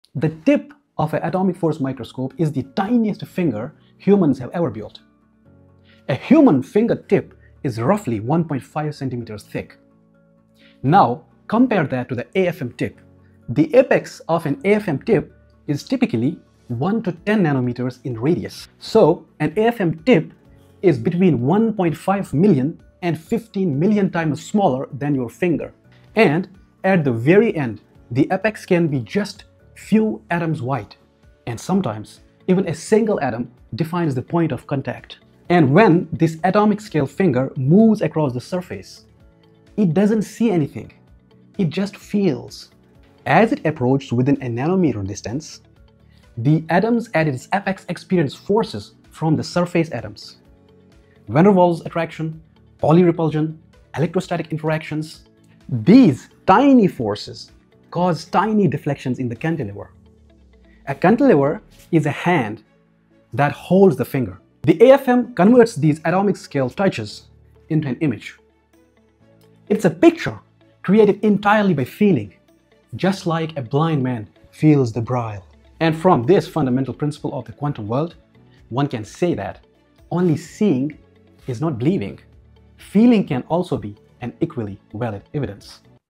Touching Atoms with a Tiny Finger
See how scientists sense forces a billion times smaller than anything we can feel.